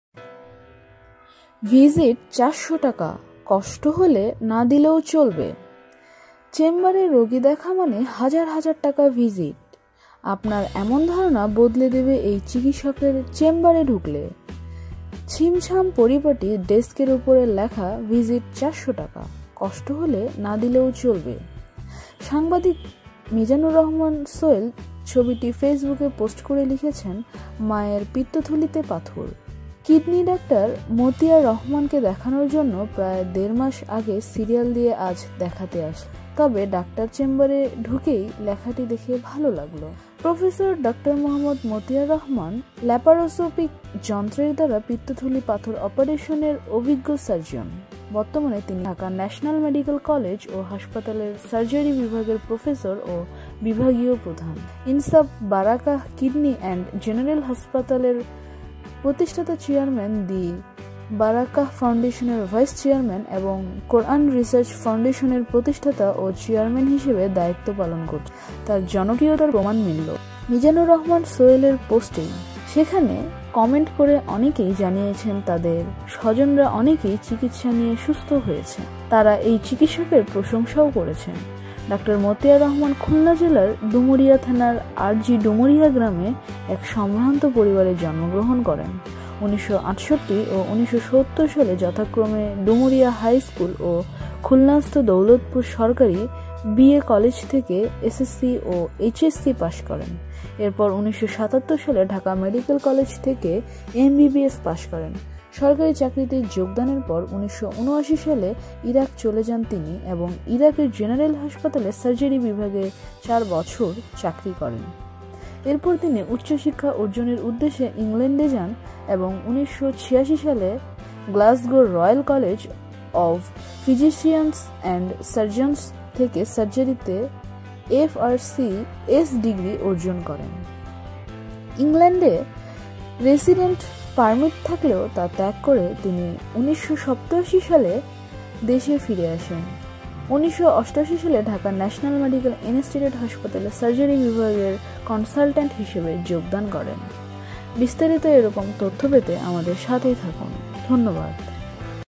‘ভিজিট ৪০০ টাকা, কষ্ট হলে না দিলেও চলবে।’
প্রফেসর ডা. মো. মতিয়ার রহমান, ল্যাপারোসকপিক যন্ত্রের দ্বারা পিত্তথলি পাথর অপারেশনের অভিজ্ঞ সার্জন। বর্তমানে তিনি ঢাকা ন্যাশনাল মেডিকেল কলেজ ও হাসপাতালের সার্জারি বিভাগের প্রফেসর ও বিভাগীয় প্রধান, ইনসাফ বারাকাহ কিডনি অ্যান্ড জেনারেল হাসপাতালের প্রতিষ্ঠাতা চেয়ারম্যান, দি বারাকাহ ফাউন্ডেশনের ভাইস চেয়ারম্যান এবং কোরআন রিসার্চ ফাউন্ডেশনের প্রতিষ্ঠাতা ও চেয়ারম্যান হিসেবে দায়িত্ব পালন করছেন।

তার জনপ্রিয়তার প্রমাণ মিলল, মিজানুর রহমান সোহেলের পোস্টেই। সেখানে কমেন্ট করে অনেকেই জানিয়েছেন, তাদের স্বজনরা অনেকেই চিকিৎসা নিয়ে সুস্থ হয়েছেন। তারা এই চিকিৎসকের প্রশংসাও করেছেন।

খুলনা জেলার ডুমুরিয়া থানার আরজি-ডুমুরিয়া গ্রামে এক সম্ভ্রান্ত পরিবারে জন্মগ্রহণ করেন। ১৯৬৮ ও ১৯৭০ সালে যথাক্রমে ডুমুরিয়া হাই স্কুল ও খুলনাস্থ দৌলতপুর সরকারি বিএম কলেজ থেকে এসএসসি ও এইচএসসি পাস করেন। এরপর ১৯৭৭ সালে ঢাকা মেডিকেল কলেজ থেকে এমবিবিএস পাস করেন।